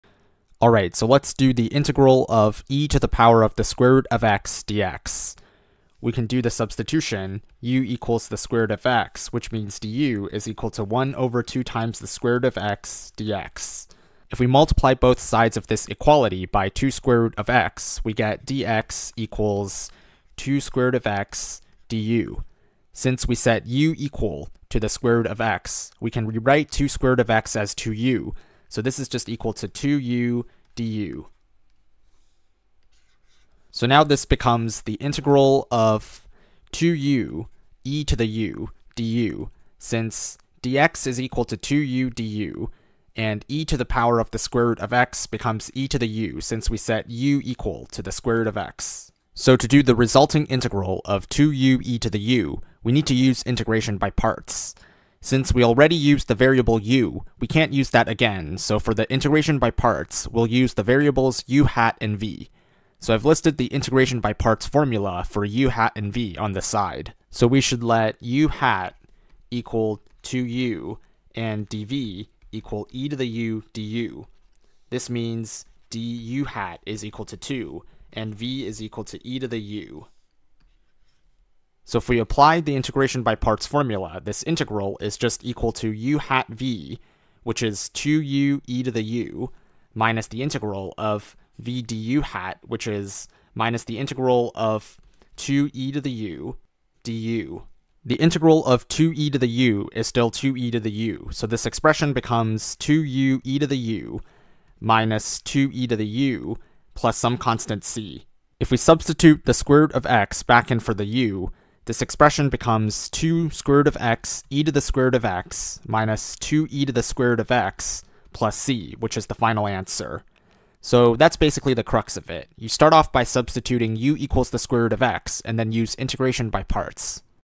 Integral of e^(sqrt(x))
We go over the integral of e^(sqrt(x)) and the u substitution to apply and how to do the subsequent integration by parts